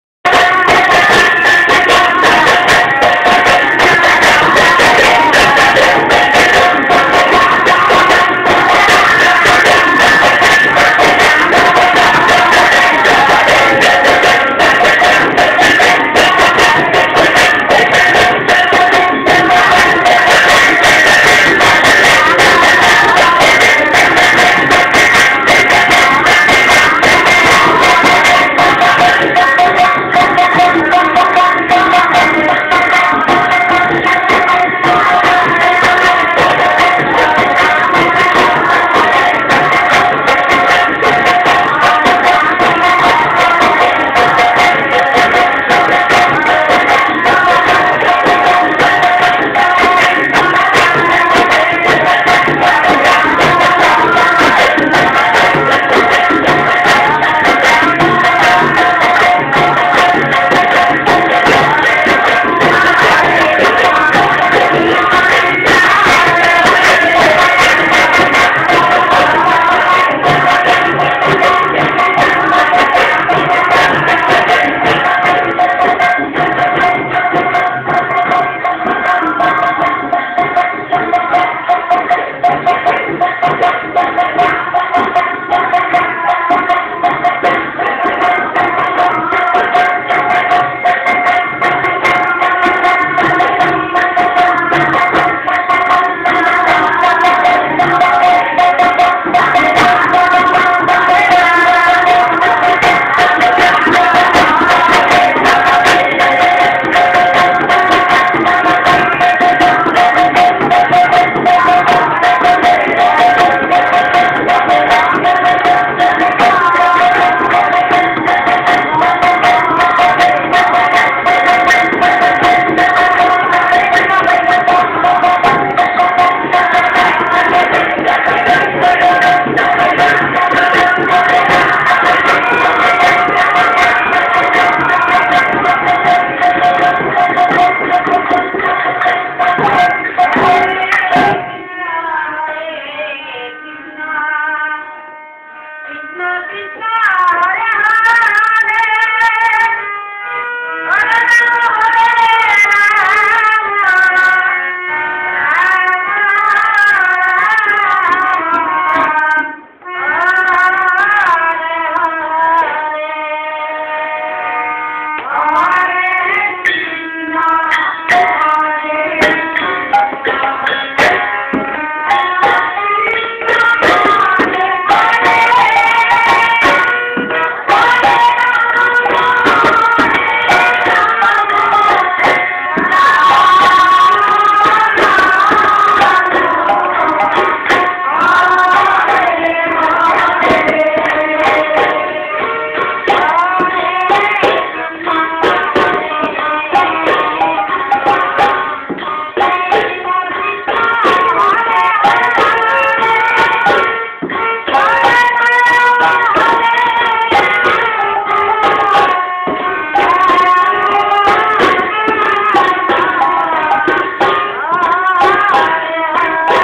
Kirtan (5/2/16), Mohaprovur akhara, Ghatia, Habiganj, Bangladesh.
Kirtan (5/2/16 tarikh Kanti Bondhu Brommocharir agomon upolokshe ), Mohaprovur akhara, Ghatia, Habiganj, Bangladesh. By- Brajendra Kumar Singha, Onontopur , Habiganj ( Baghbari, Kamalganj, Moulvibazar ) , Bangladesh.